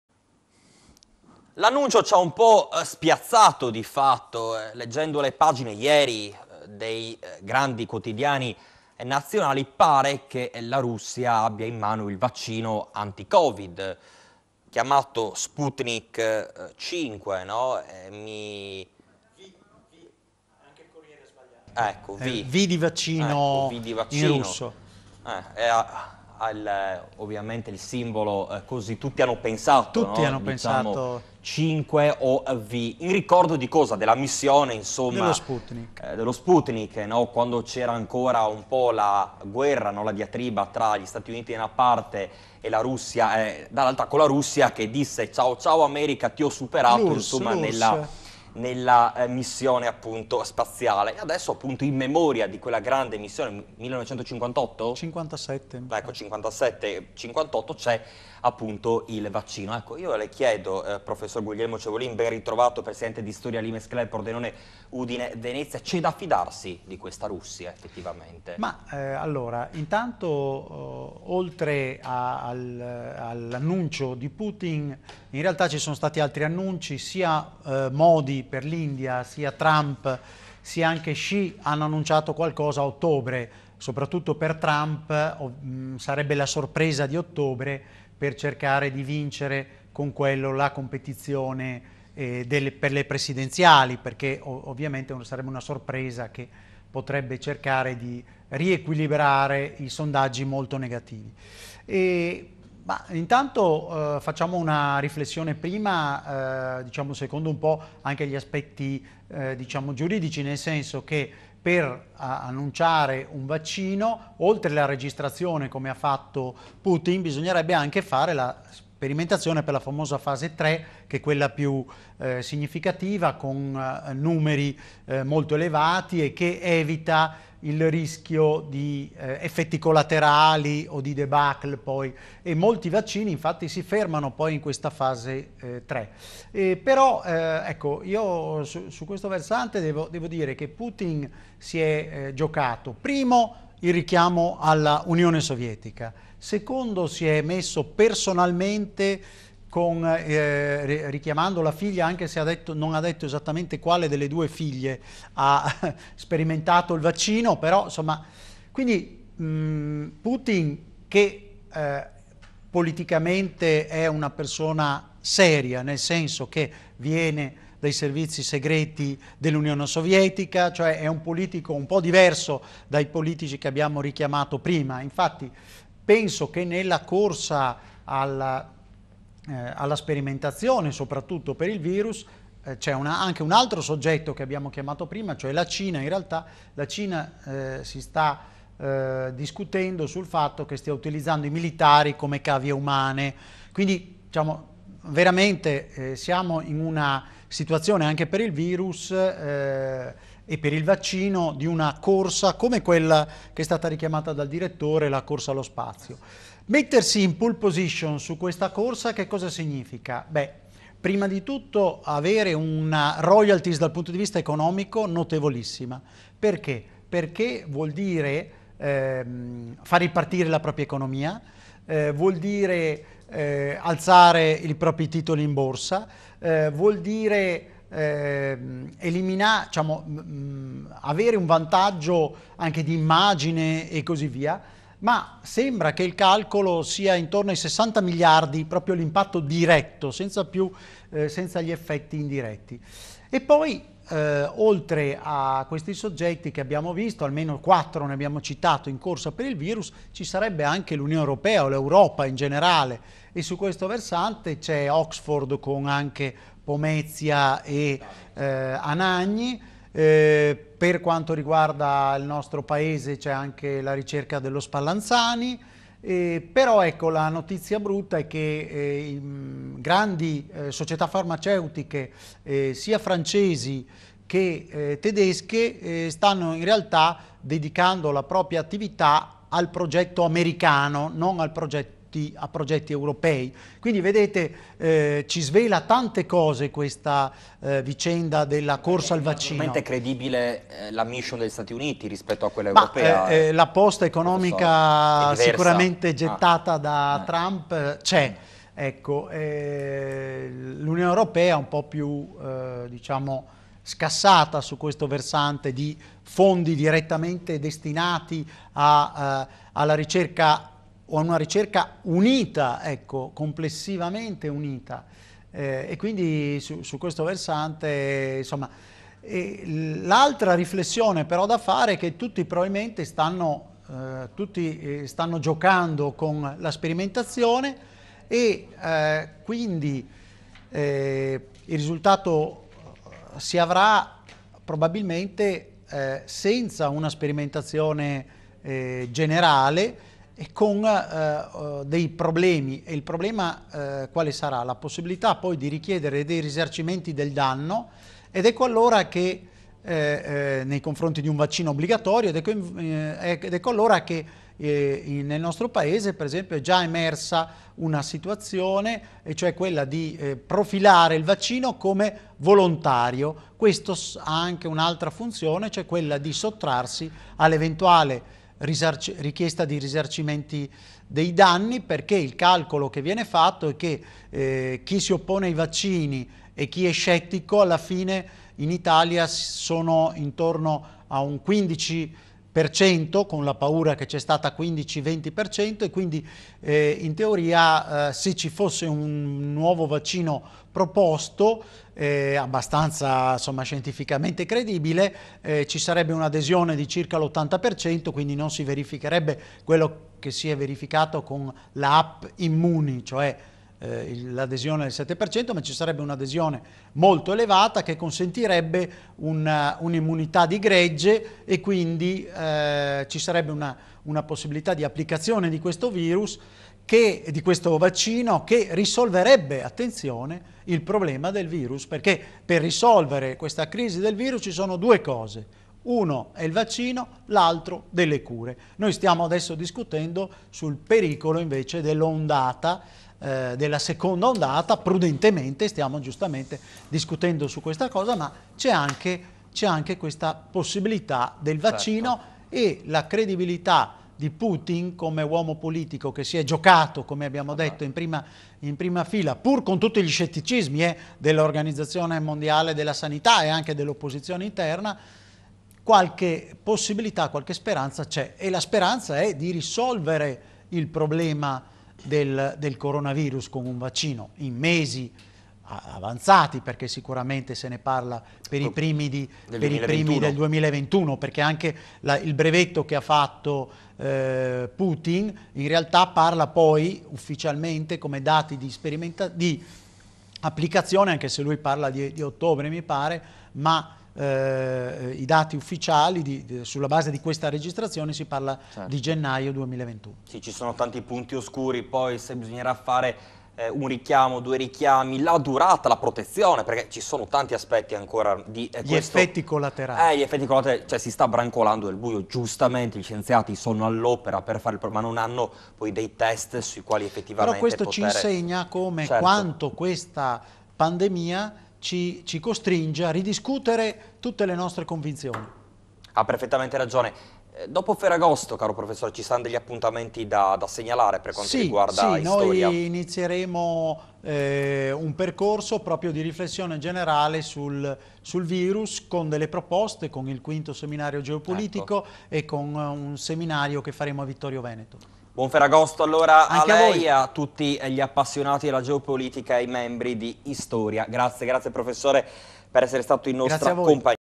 Competizione geopolitica per il vaccino
Il Dott. Alberto Comisso Direttore di Telepordenone intervista il Prof. Avv. Guglielmo Cevolin coordinatore del LiMes Club Pordenone Udine Venezia sulla corsa al vaccino per il Covid 19 al telegiornale di venerdì 14 agosto 2020.